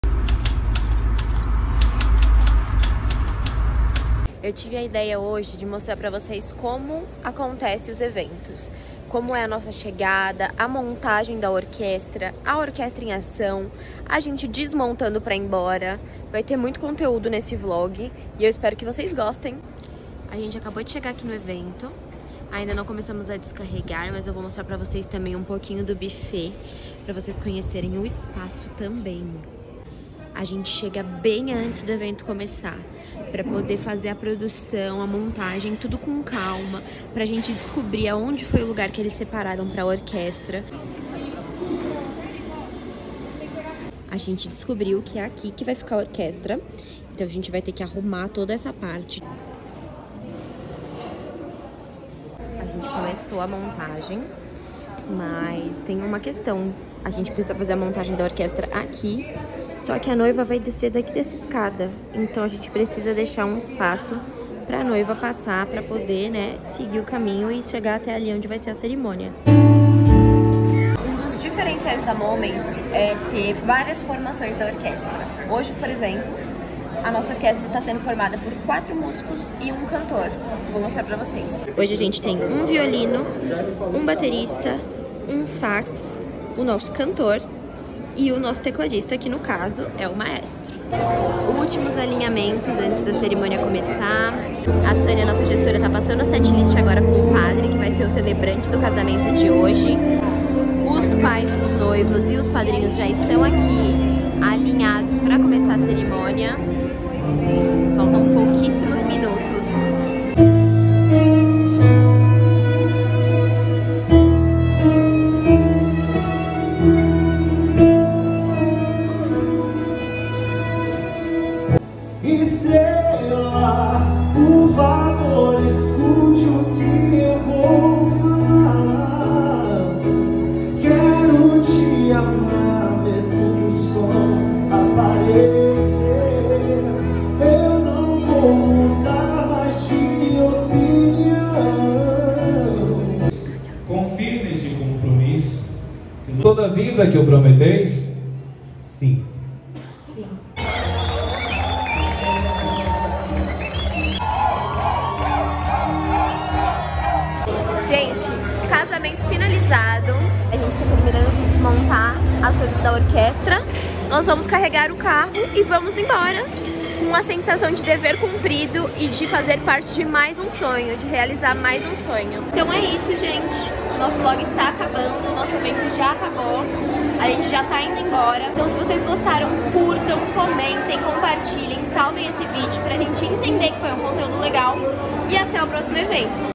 Vlog #1 - Casamento com tema Sertanejo
Sejam bem-vindos ao primeiro vlog do nosso canal!!!

Nesse vídeo vamos compartilhar com vocês um pouco da nossa rotina em dias de evento!

Assiste o vídeo até o final que vai valer muito a pena!

Entre em contato conosco e peça já seu orçamento!